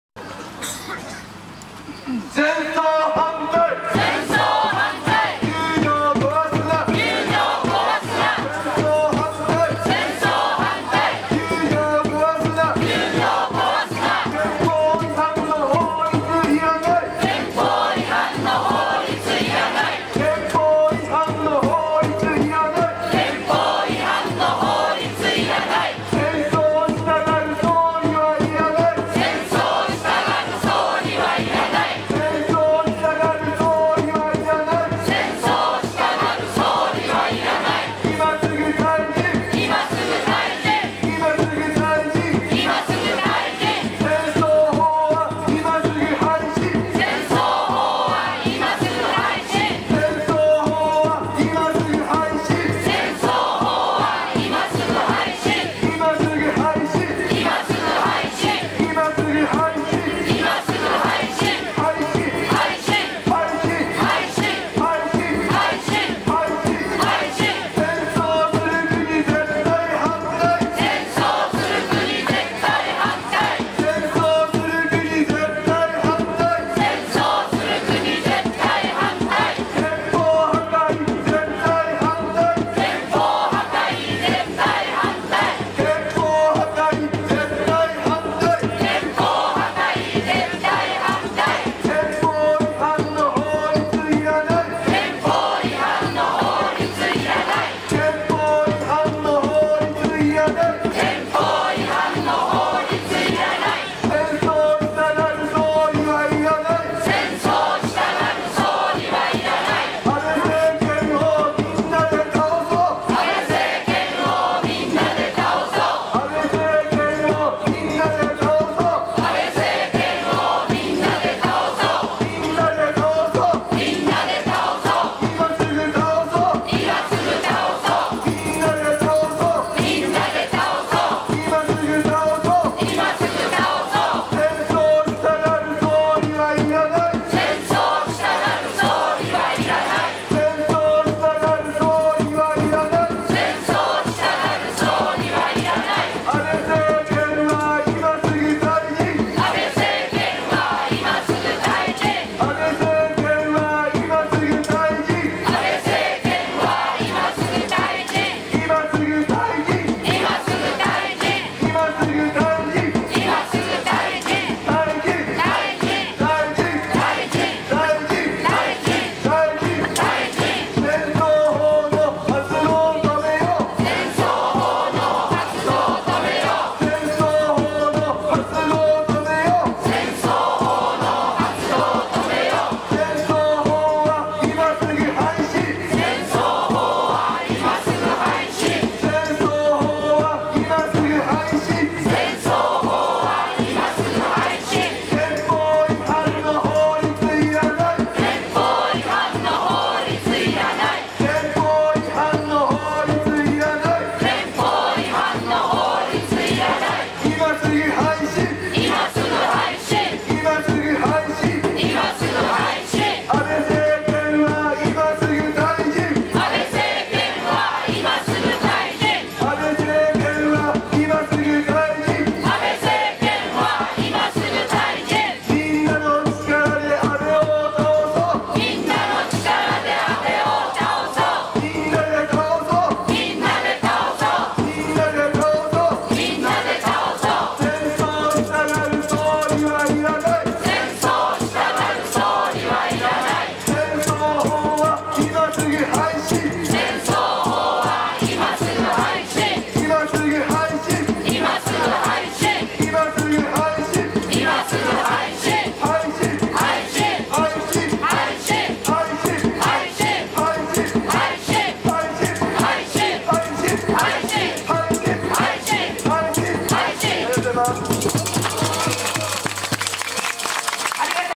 2015.10.19「…戦争法廃止！…国会正門前集会」: コール (2)【8/14】
「私たちはあきらめない！戦争法廃止！安倍内閣退陣！国会正門前集会」

日時：2015年10月19日（月）18:30 ～
場所：国会議事堂正門前
呼びかけ：戦争させない・９条壊すな！総がかり行動実行委員会

〈毎月１９日は、国会前に集まろう！〉

９月１９日未明、雨の国会周辺を取り巻く多数の市民が叫ぶ「戦争法案廃案！」のコールのなか、参議院本会議で安倍政権と与党自民党・公明党などによる戦争法制の強行採決が行われました。しかし、強行採決されたとはいえ、私たちの「戦争反対！」の闘いは終わらず、ひきつづき毎月「１９日行動」として継続されます。私たちはあきらめません。再度の結集を呼びかけます。国会正門前へ！